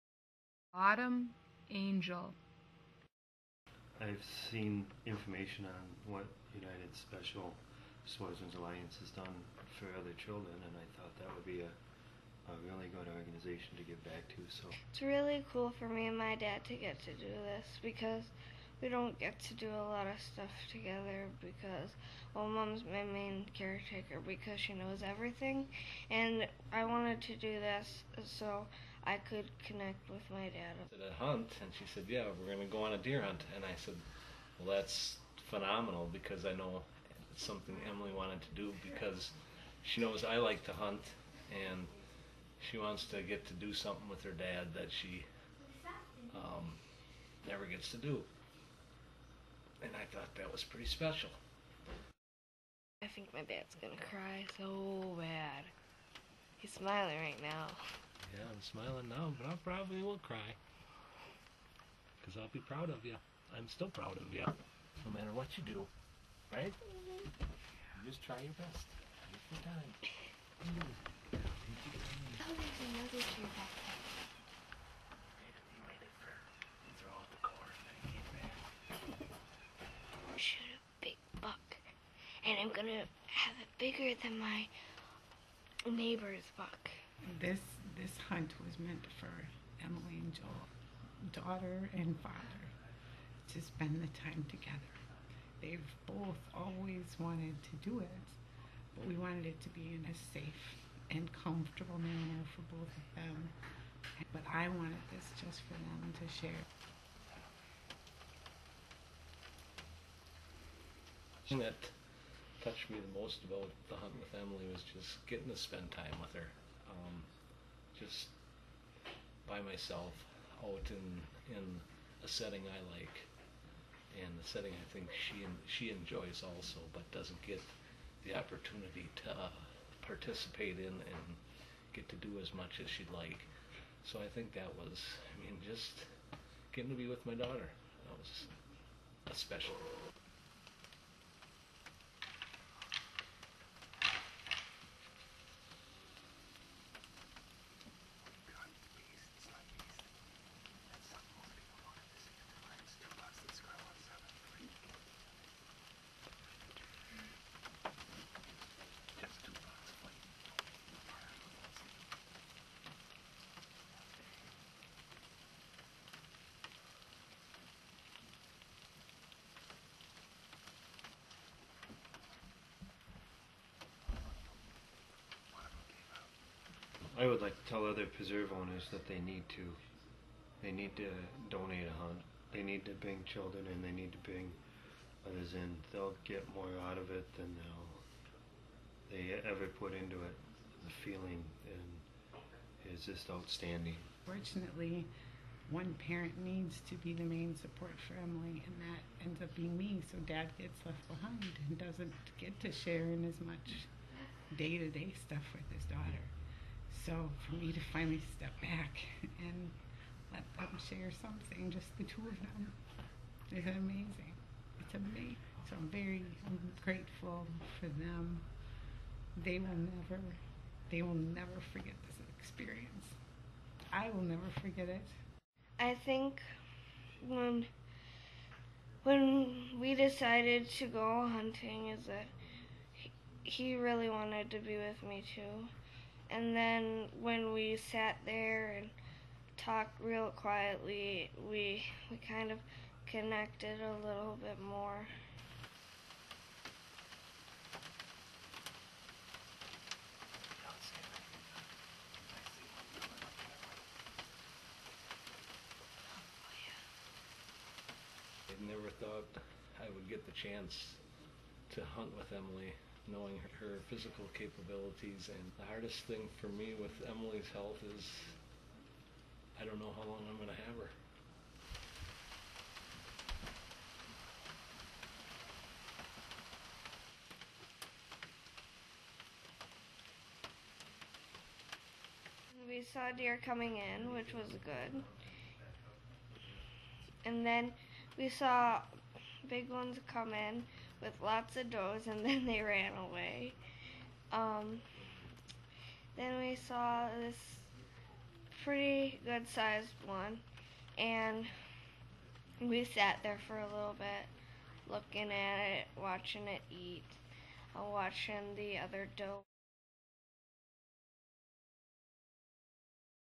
Autumn Angel (part 1)
12 year old girl with a life threatening illness from Wisconsin harvested a dandy of a buck.    Make sure you watch the conclusion in Part 2.